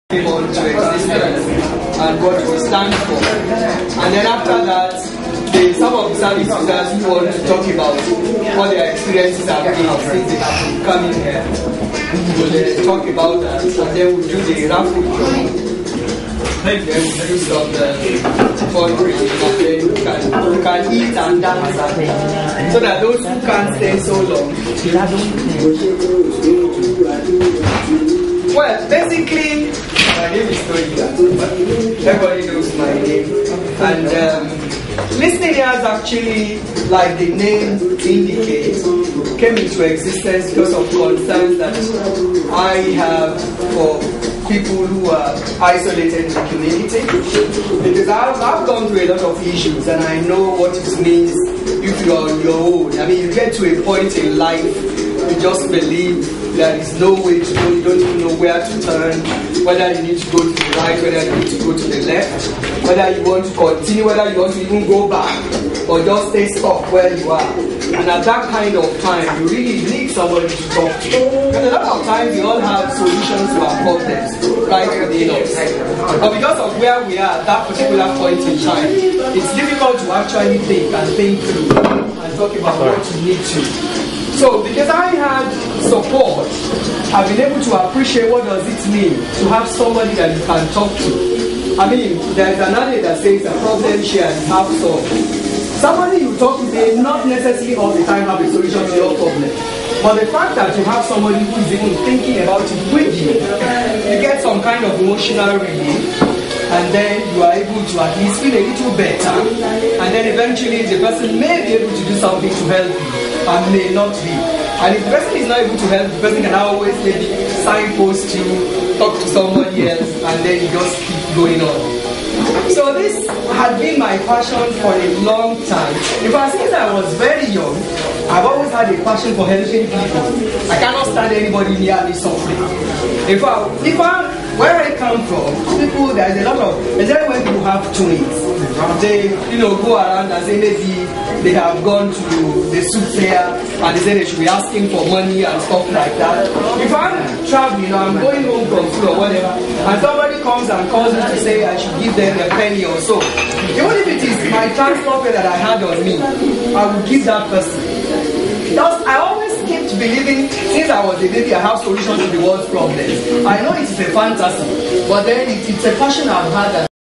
Listening Ears Christmas Party 2008 (Part1)
Toyin Ogunbanjo making a presentation at Listening Ears Christmas Party 2008